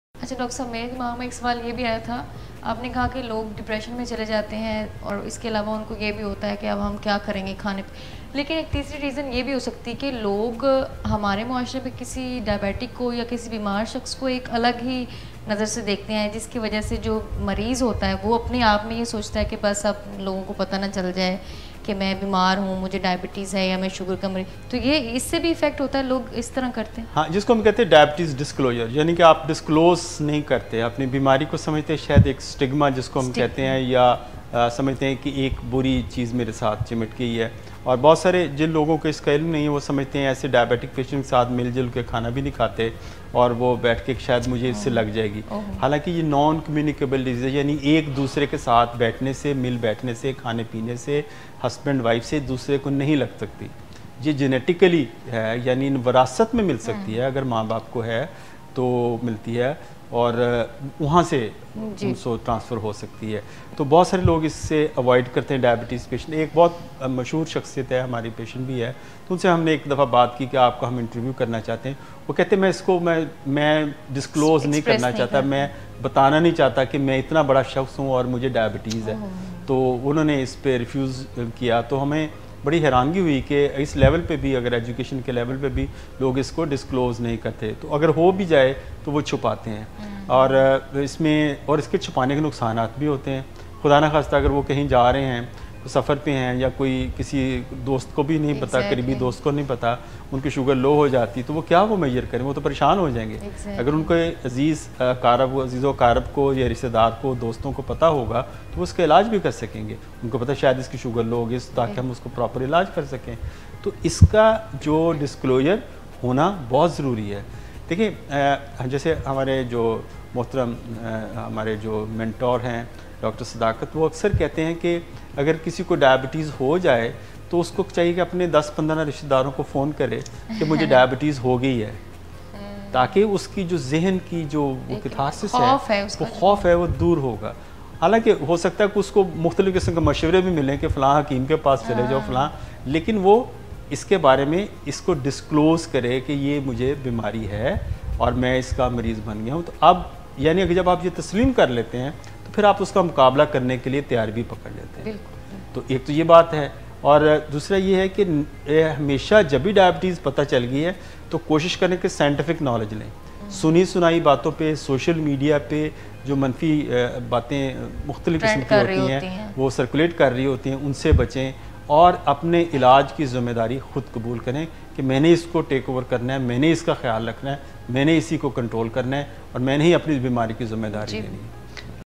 لوگ یہ بتانے میں شرم کیوں محسوس کرتے ہیں کہ انہیں ذیابیطس ہے؟
◆ For Treatment Appointments:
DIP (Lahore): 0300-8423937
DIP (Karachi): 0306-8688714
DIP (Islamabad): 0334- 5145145